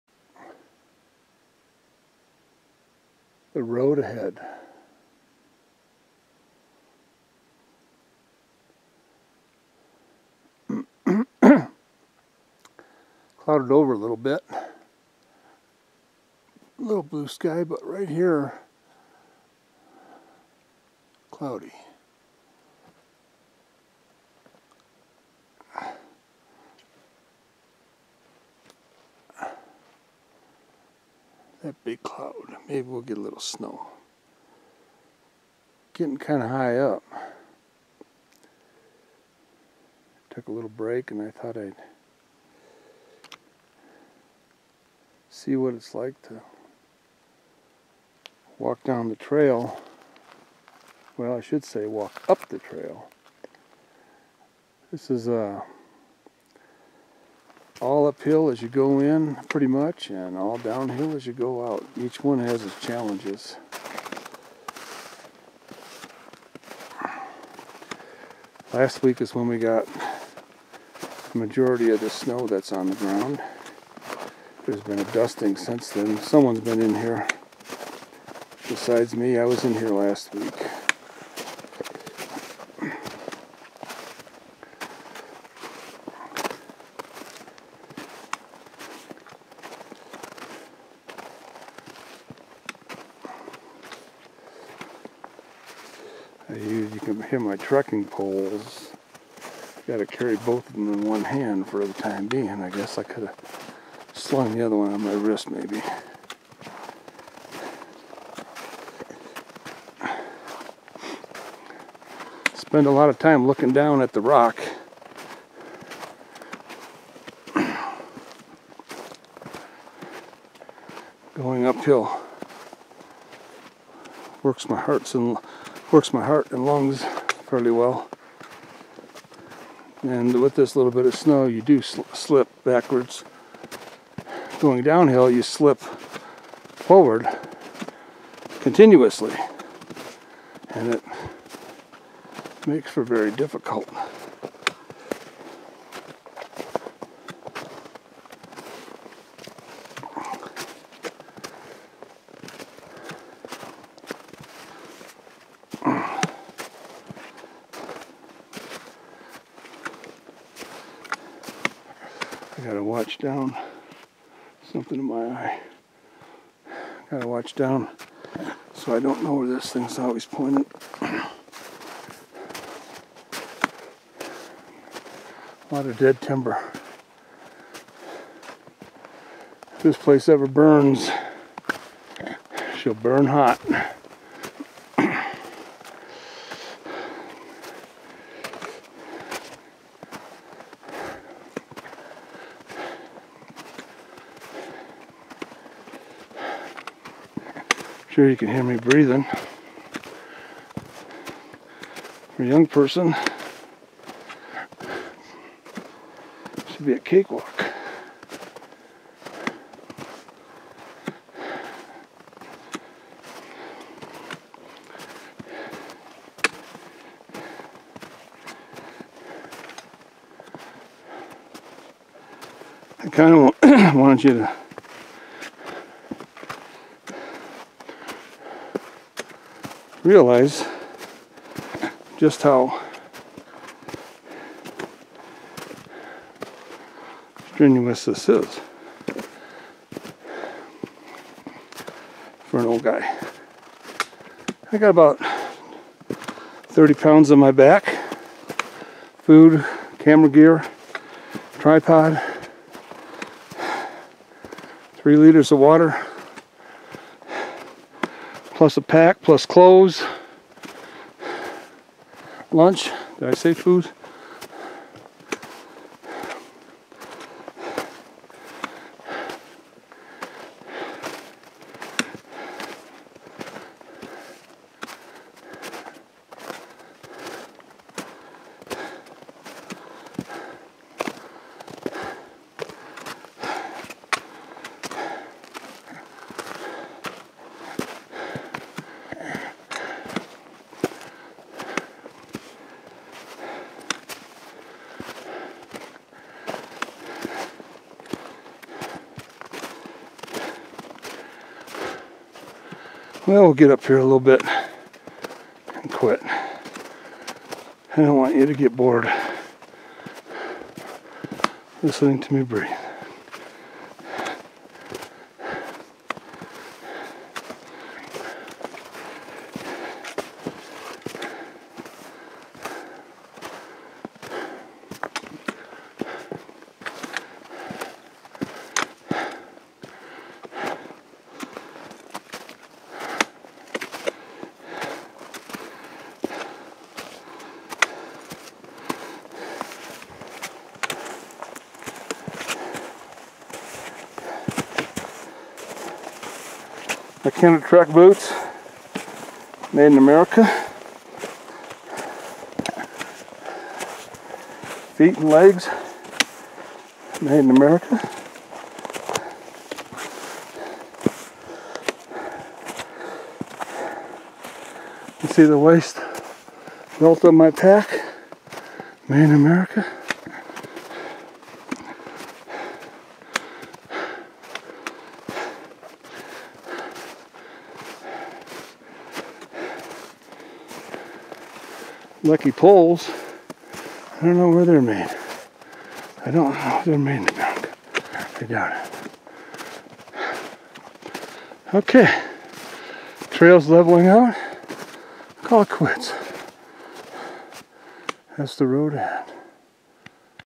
Willow Lake Hike #3
Day hike into Willow Lake. Wyoming's Big Horn Mountains, Cloud Peak Wilderness.